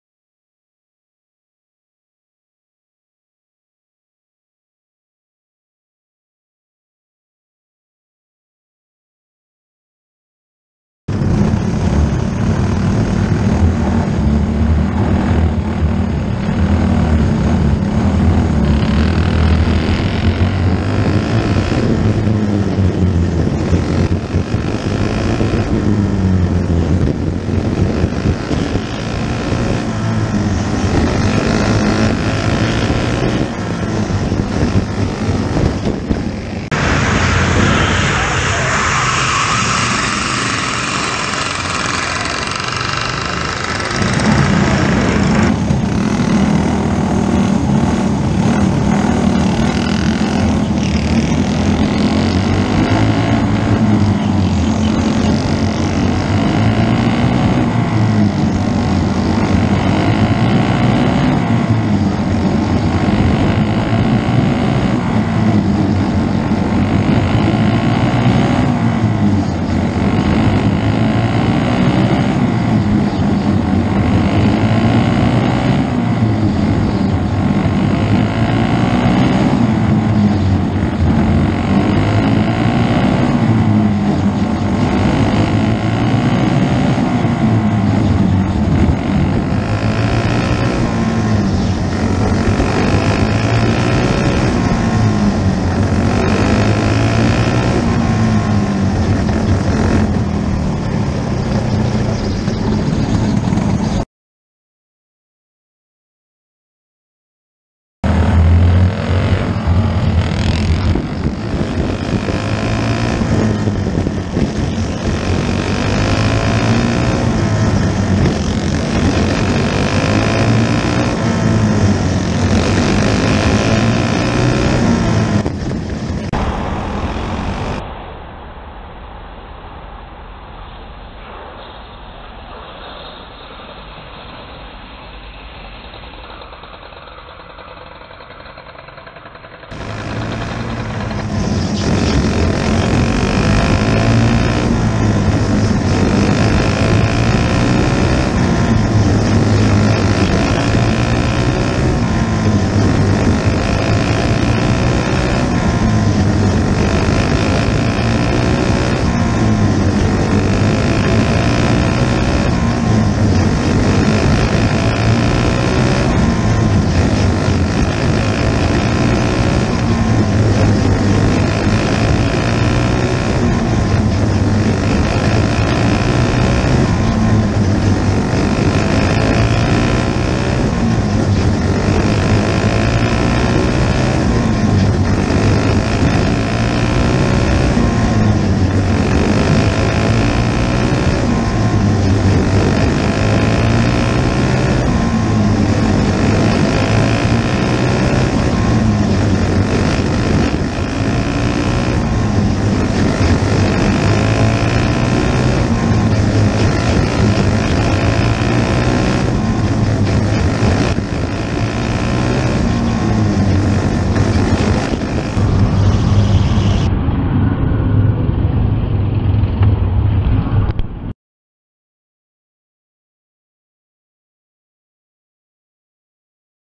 Concrete Challenge 2021
Box Stock Heat and Feature events from the GoPro onboard KC Sparks' Outlaw wing kart.